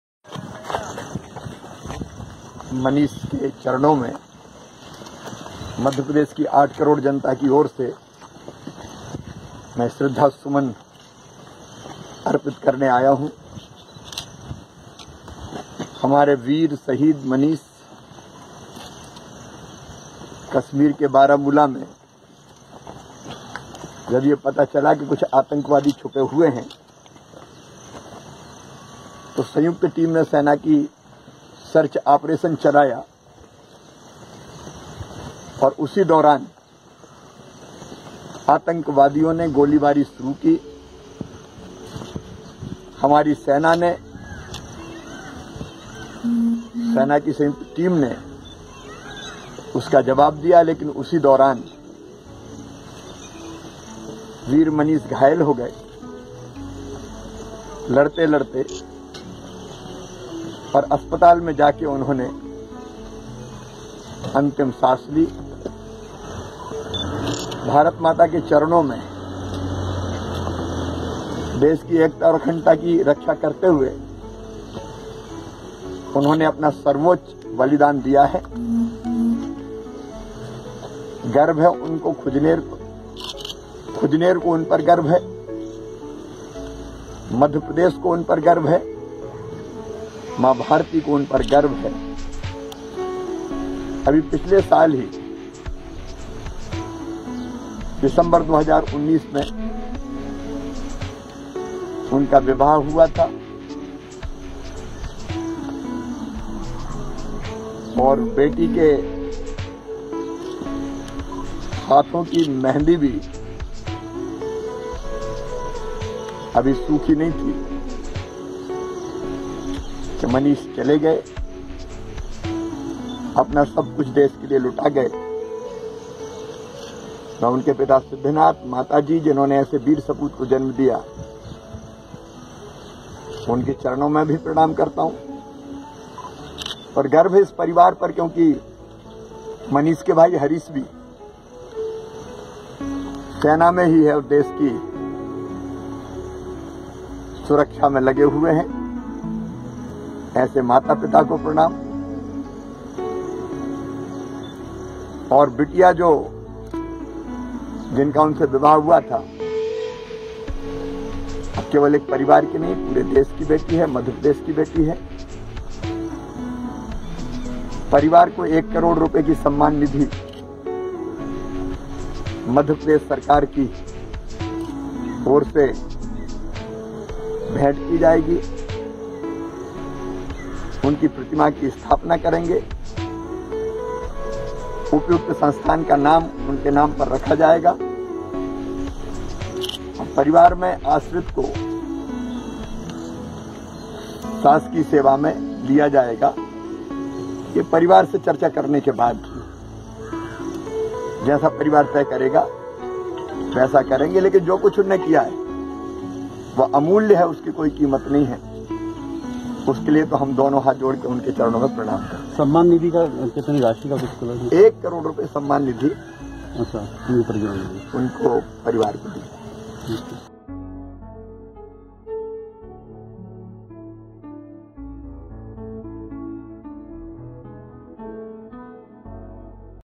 खुजनेर राजगढ़ के शहीद सैनिक मनीष कार्पेंटर को शिवराज सिंह की श्रद्धांजलि देखे विडिओ
मनीष कारपेंटर

manish carpenter